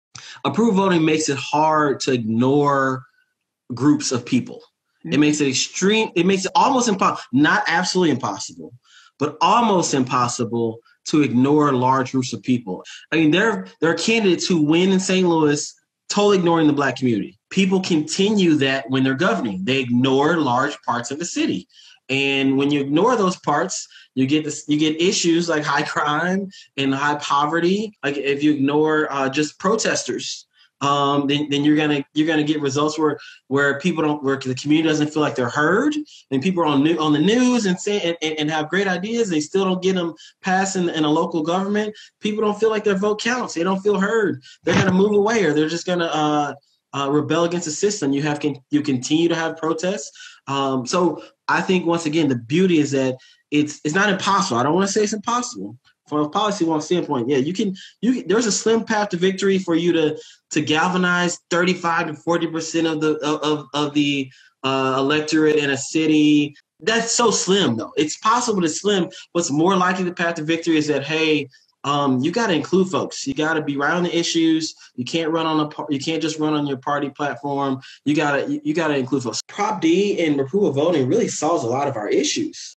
Candidates Can't Ignore Voters under Approval Voting
Michael Butler, the Recorder of Deeds for the City of St. Louis, explains how approval voting incentivizes candidates and elected officials to consider the needs of EVERY community.